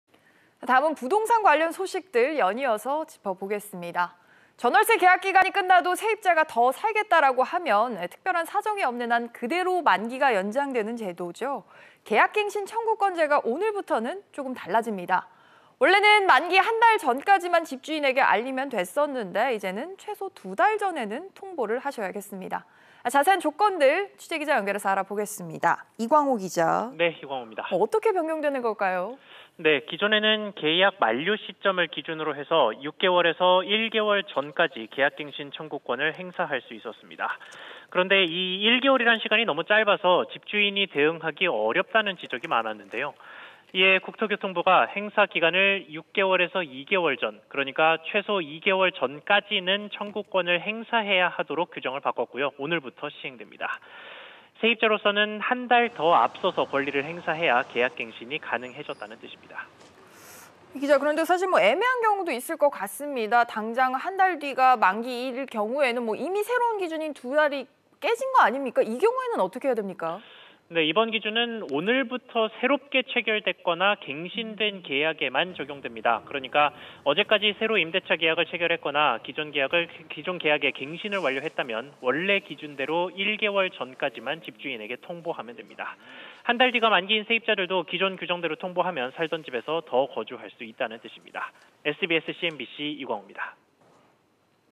계약갱신청구권 만기 ‘두 달 전 통보’…기존 계약은?
[앵커]

전·월세 계약 기간이 끝나도 세입자가 더 살겠다고 하면 특별한 사정이 없는 한 그대로 만기가 연장되는 계약갱신청구권제가 오늘(10일)부터 약간 바뀝니다.

원래는 만기 한 달 전까지만 집주인에게 알리면 됐었는데, 이젠 최소 두 달 전에는 통보해야 합니다.

자세한 조건 알아보겠습니다.

이광호 기자, 자세한 변경 내용 전해주시죠.

[기자]

기존에는 계약 만료 시점을 기준으로 6개월에서 1개월 전까지 계약갱신청구권을 행사할 수 있었습니다.